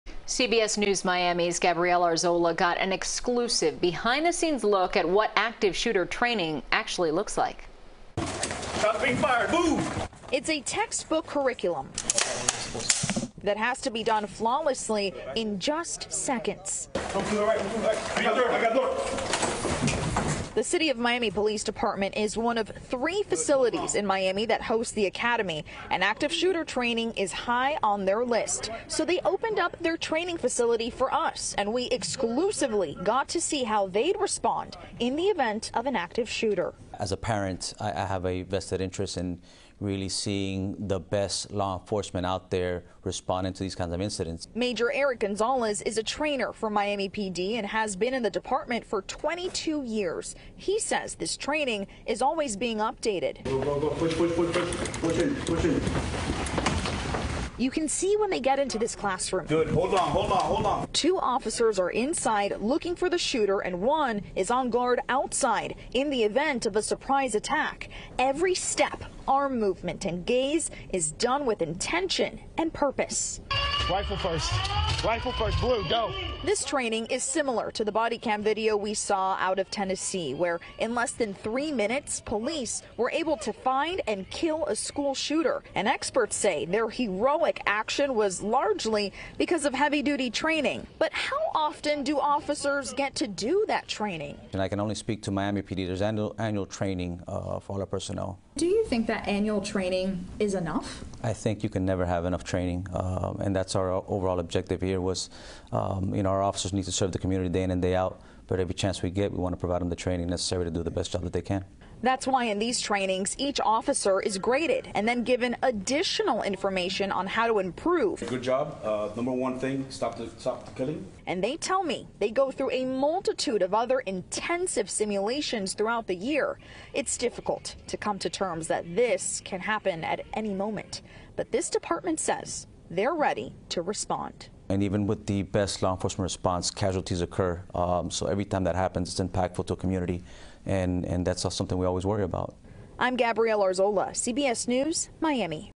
A behind-the-scenes look at active shooter drill by Miami police
Days after the latest school shooting in Tennessee, CBS News Miami takes an exclusive behind-the-scenes look at active shooter training by City of Miami police.